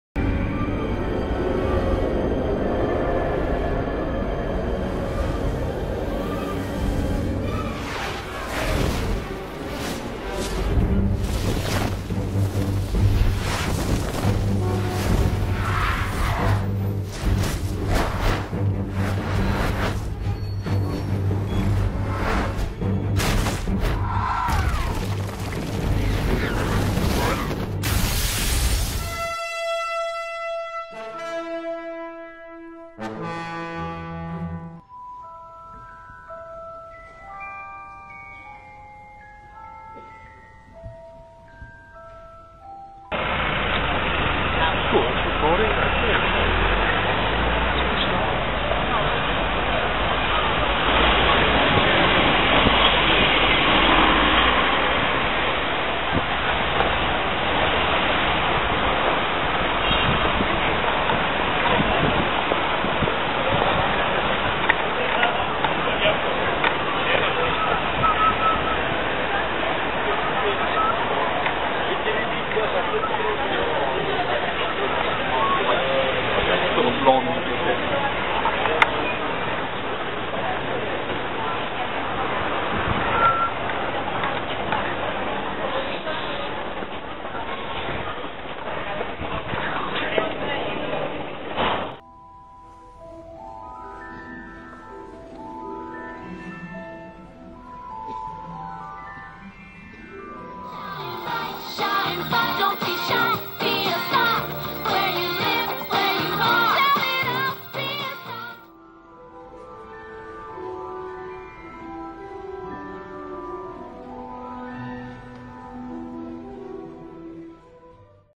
43 Things - Visit Harry Potter film locations - Great Newport St
On Wednesday the 19th February we visited Great Newport St, London. Great Newport St was the site of Diagon Alley at the start of Harry Potter and the Half-Blood Prince.